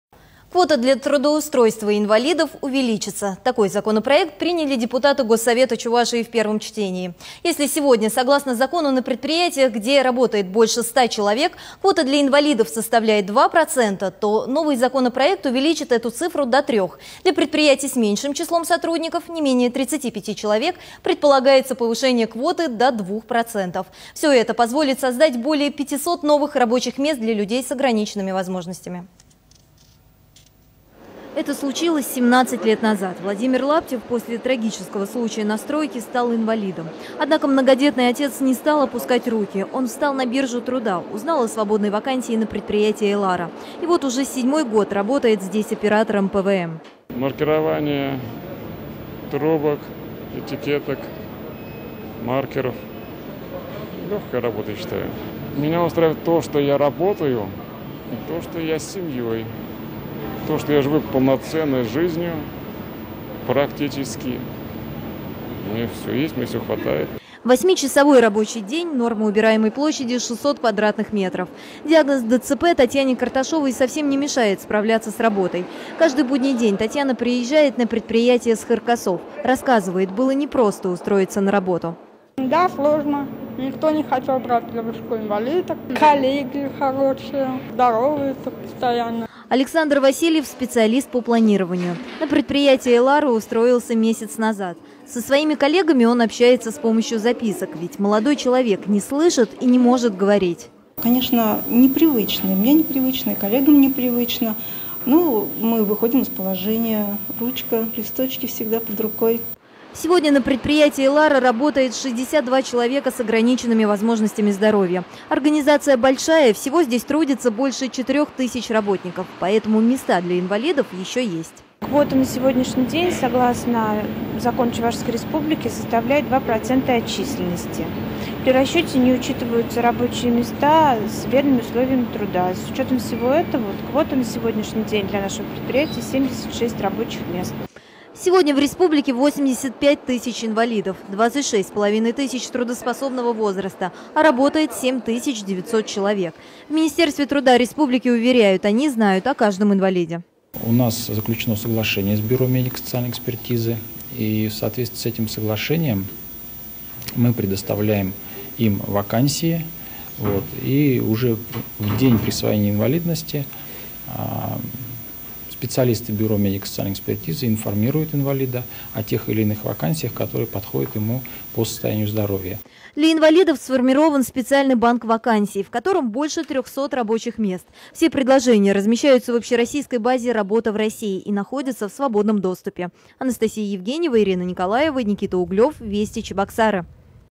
Предприятия республики трудоустраивают инвалидов и создают для них комфортные условия
Квота для трудоустройства инвалидов увеличится. Такой законопроект приняли депутаты Госсовета Чувашии в первом чтении.